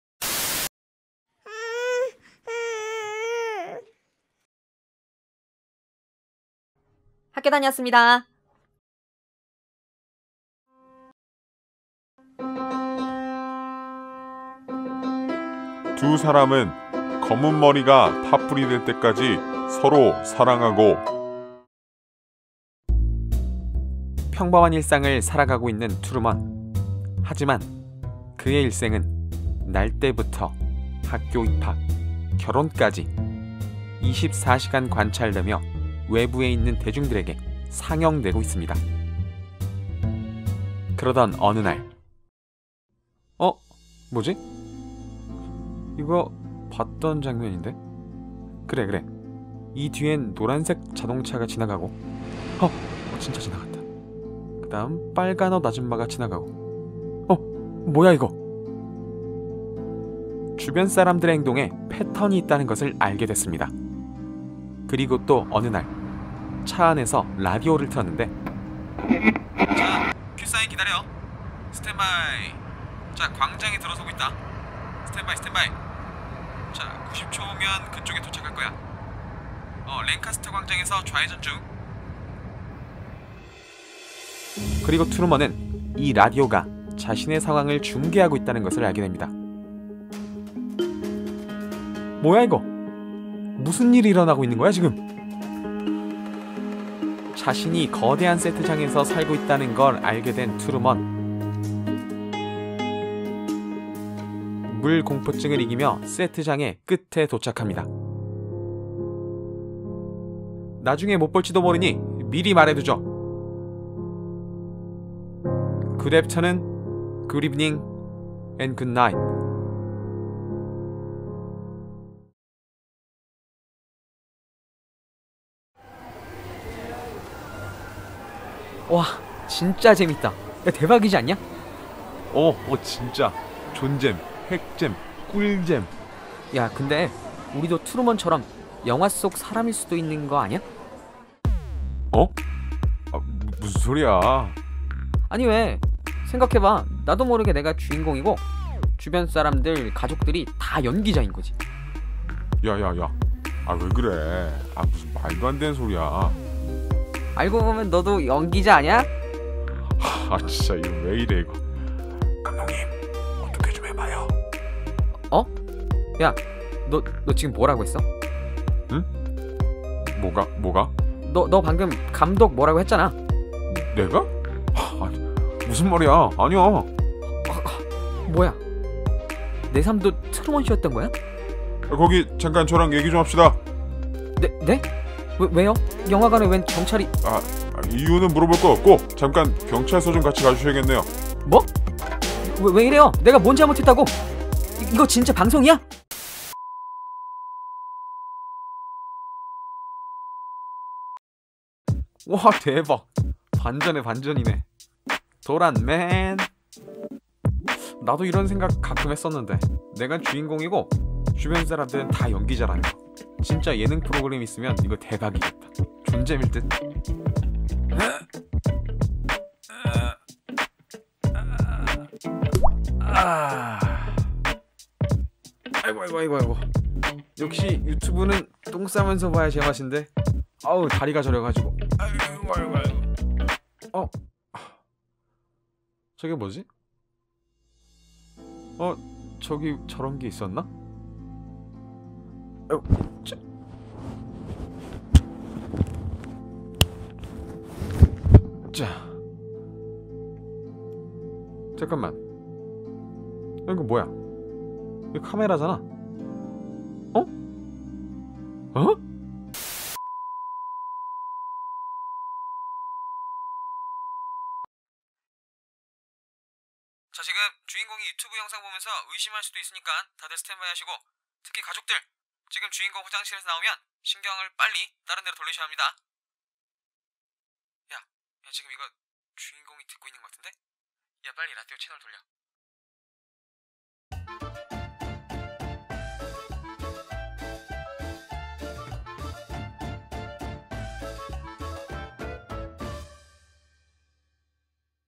[반전주의] 당신의 삶이 24시간 관찰되고 있다.｜빨간토마토
아직 트루먼쇼를 안보신 분 계시다면
꼭 ~~ 보셔아합니다. :) 진짜 존잼! 핵잼! 꿀잼!

*마지막 반전을 못들으신 분들은 4:27초로

편집 : 노랑파
섬네일 : 개동